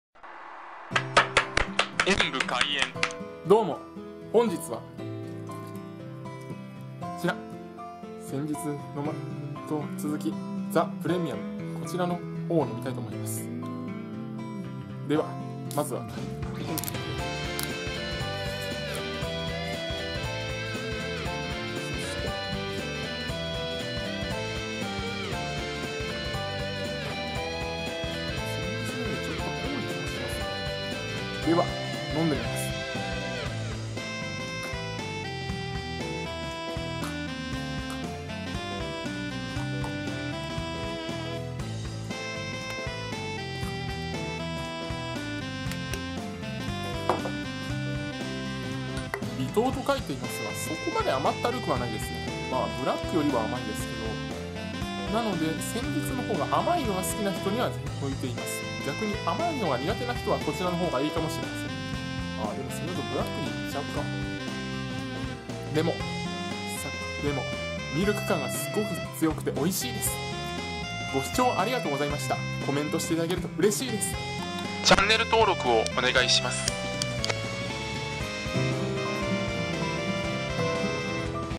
赤い彗星の贈り物２ GEORGIA プレミアムボトル缶コーヒーを飲んでみた
GEORGIA　プレミアムボトル缶コーヒーを飲んでみました。
赤い彗星様、本当にありがとうございます！！